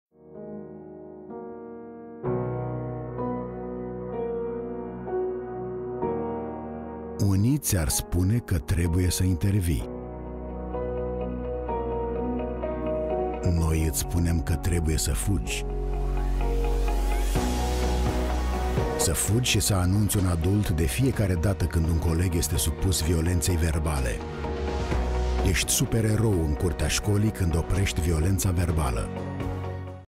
Ești erou în curtea școlii când oprești violența verbală! (profesor-elev)
Spotul a fost dezvoltat in cadrul campaniei împotriva violenței în școli derulata de UNICEF și Ministerul Educației și Cercetării Științifice (MECS).

Scopul campaniei este creșterea nivelului de informare și conștientizare la nivel național și local în privinţa formelor și consecințelor violenței verbale în școală. Campania se va desfășura în intervalul octombrie 2015 – iunie 2016 și include două spoturi video (unul care ilustrează violența verbală între colegi și unul între profesor și elev), două spoturi radio, machete pentru panotaj și campanie online. Campania încurajează copiii să raporteze cazurile de violență și, pentru că ei își ajută colegii, vor fi numiţi „supereroi”.

Potrivit datelor MECS, în anul școlar 2014 /2015, au fost raportate la nivel național 18.793 de cazuri de violență în școală, număr în scădere raportat la anul școlar anterior, dintre aceste cazuri, 3397 fiind asociate violenței verbale. Conform studiului global UNICEF pe tema violenței din anul 2014, în România, 41% dintre copii erau intimidați și hărțuiți de colegi la școală. Cea mai des menționată formă de violență împotriva copilului a fost cea elev-elev (certuri, conflicte, țipete, folosirea de apelative) comparativ cu elev-profesor sau profesor-elev (ironie, țipete, ofense sau jigniri). De asemenea, violența verbală este tipul de violență menționat cel mai des, deși este considerată o formă de abuz mai puțin gravă.